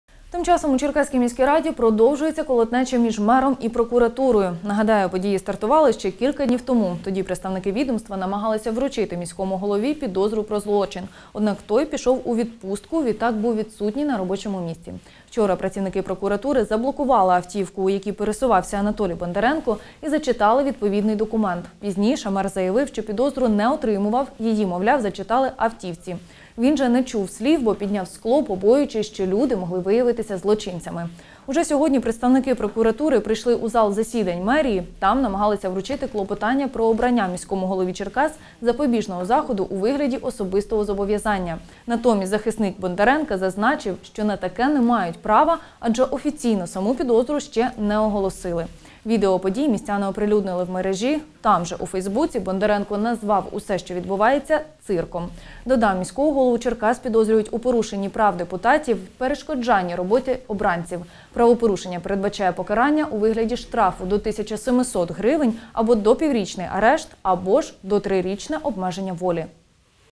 Черкаський прокурор намагався вручити мерові повідомлення про обрання запобіжного заходу
Міського голову Черкас підозрюють у порушенні прав депутатів й перешкоджання роботи обранців